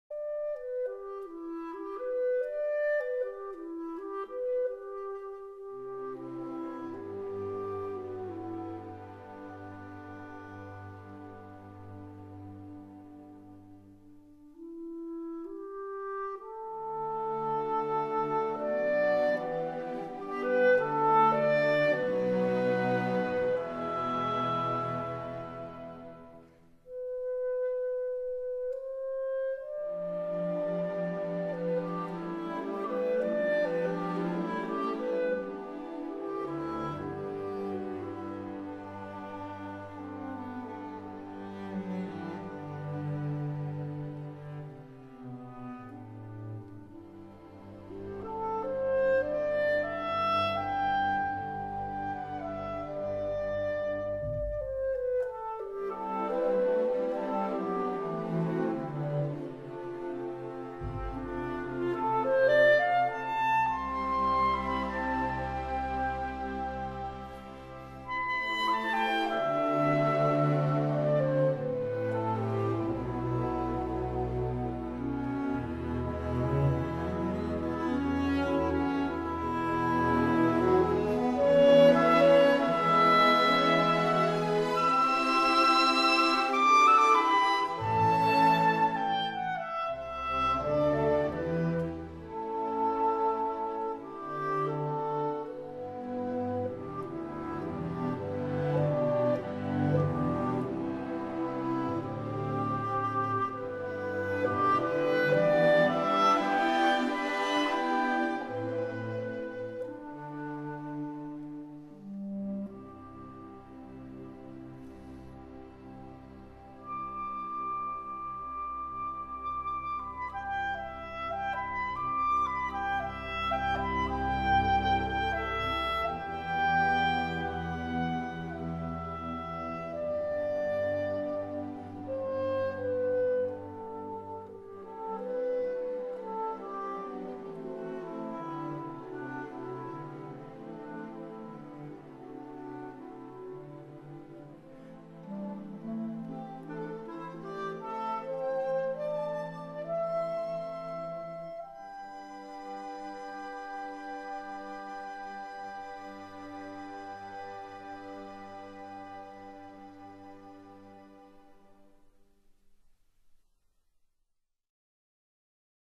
Crook Hall and Gardens, Durham
Crook Hall and Gardens at Durham using photographs taken May 2010